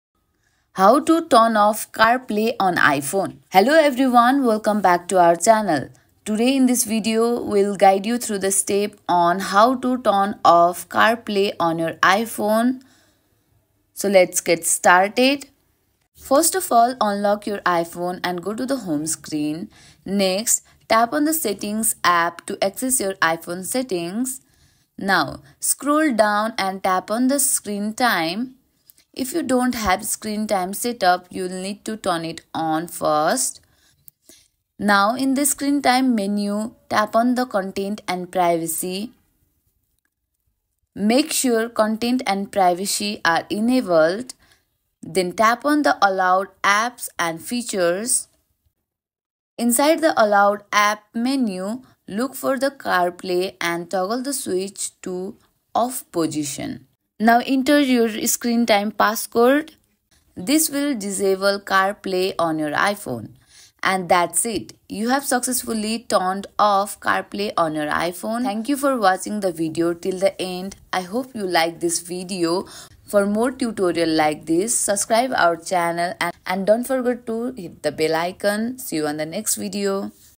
How To Turn Off CarPlay On iPhone! (2024)
In this tutorial, learn the step-by-step process to turn off CarPlay on your iPhone effortlessly in 2024.

Timestamps:
0:00 - Introduction
0:10 - Step-by-step guide to turn off CarPlay

Key points covered in the video:
- Why you might want to turn off CarPlay on your iPhone.
- How to access CarPlay settings on your iPhone.
- Step-by-step instructions to disable CarPlay.
- Tips for managing CarPlay settings for different vehicles.
- Troubleshooting tips for any issues encountered while turning off CarPlay.